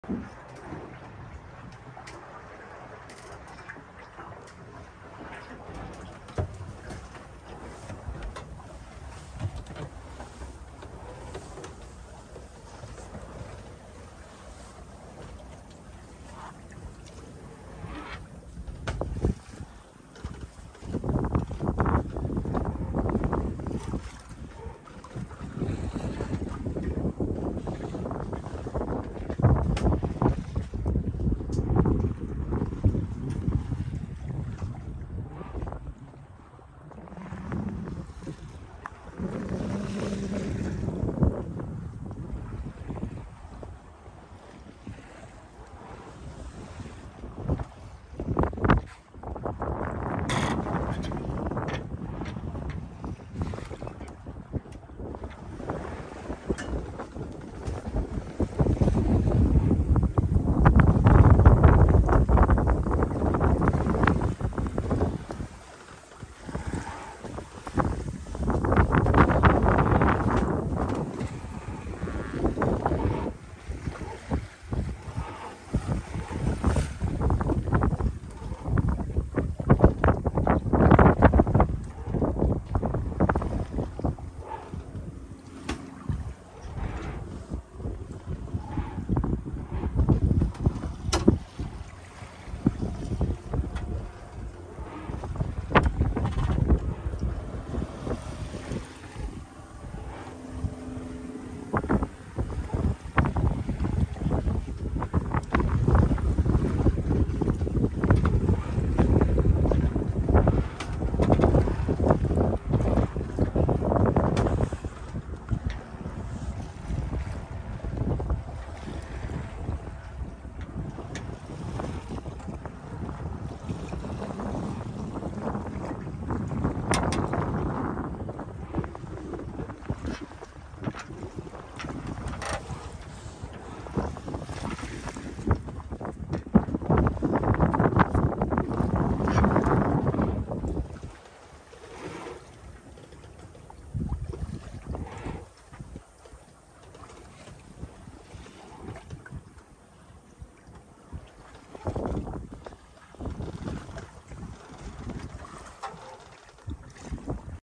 Sailing ⛵️ Bavaria 34 Cruiser Yacht Broken Bay Sydney Australia
Sailing my Bavaria 34 Cruiser Yacht in beautiful weather out in Broken Bay Pittwater Sydney Australia